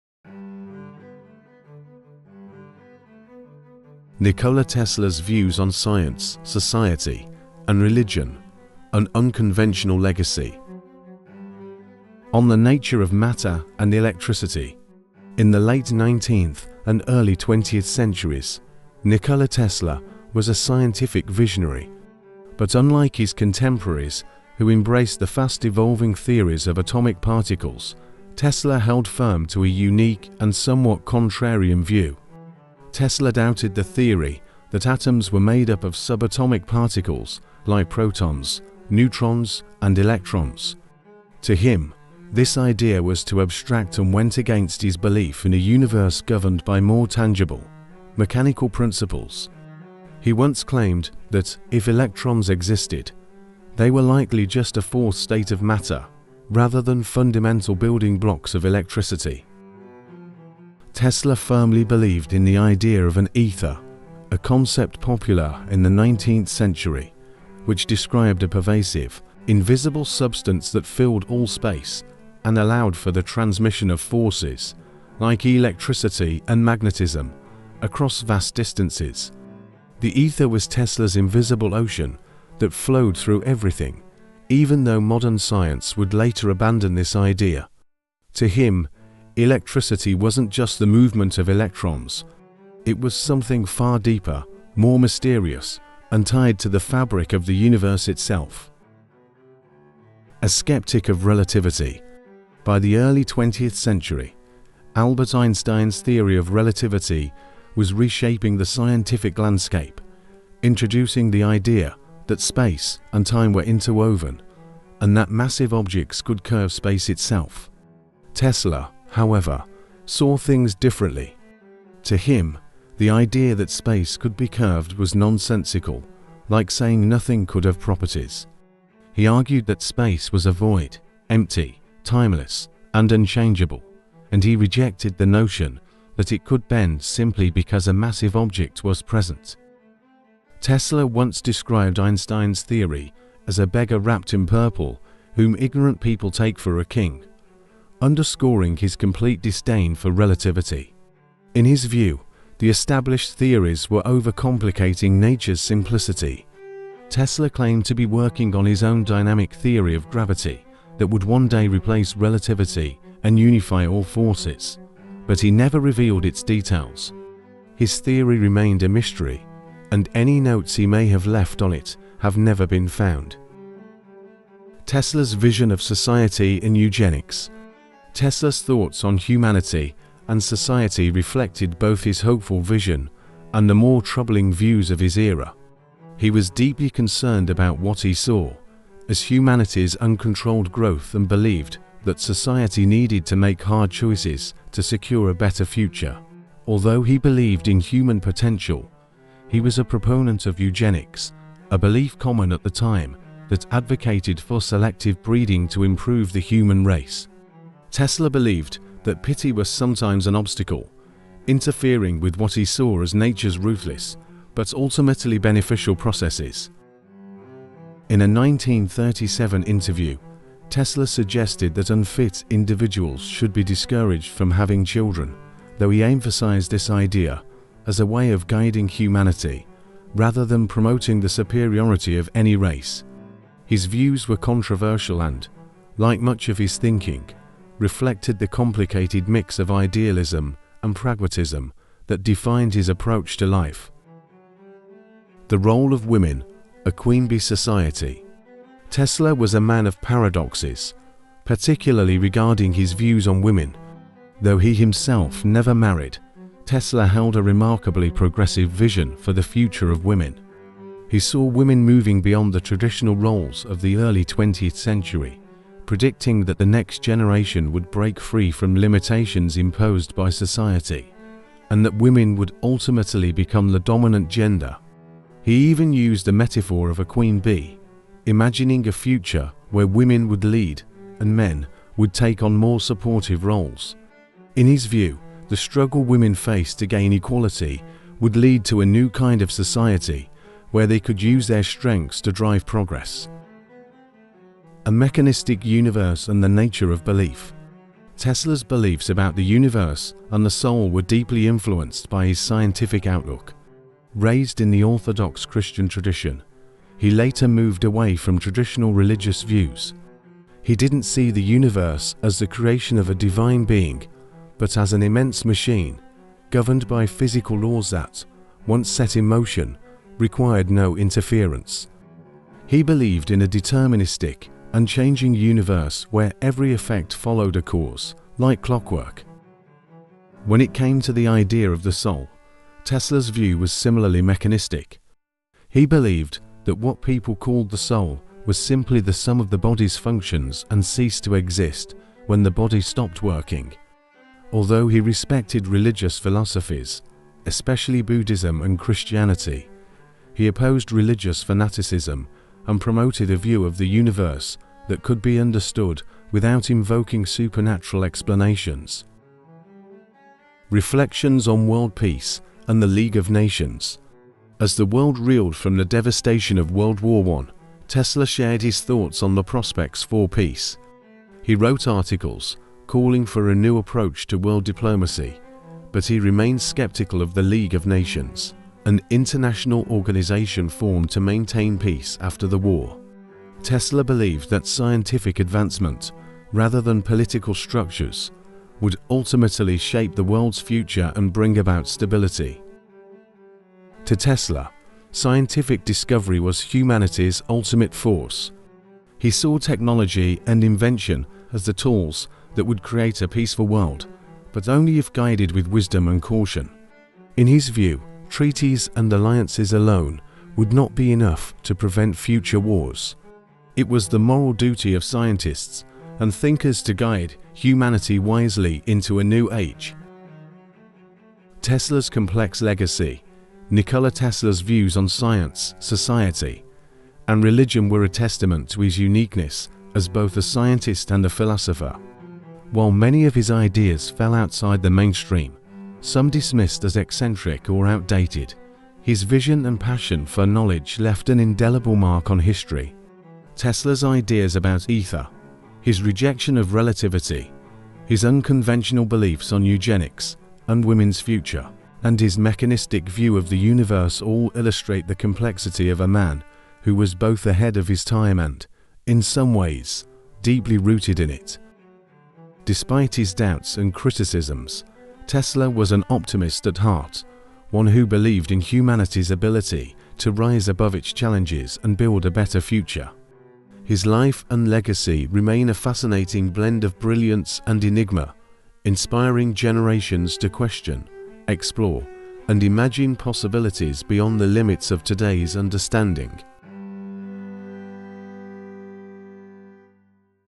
Nikola Tesla’s Views on Science, Society, and Religion: An Unconventional Legacy
Dive into the unique mind of Nikola Tesla, a visionary who challenged conventional science, society, and religion. In this video, we explore Tesla’s unconventional views on atomic theory, his skepticism of Einstein’s relativity, his belief in the ether, and his controversial stance on eugenics. Learn about Tesla's futuristic thoughts on gender roles and his belief in a deterministic universe. Join us as we uncover Tesla’s complex legacy and how his radical ideas continue to inspire and intrigue generations.